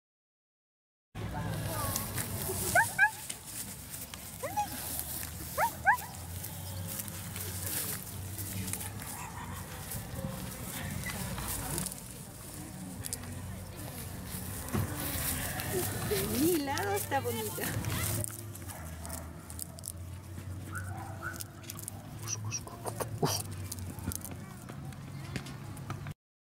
Kusi Alqu 2017 - a los 3 meses y medio - vídeo 19
La única cachorra negro y fuego de mis compañeros Danna y Yaco con tres meses y medio juega en el parque. Ya tiene todas sus vacunas y desparasitaciones. Tiene una dieta a base de pollo fresco y mucho amor de parte nuestra.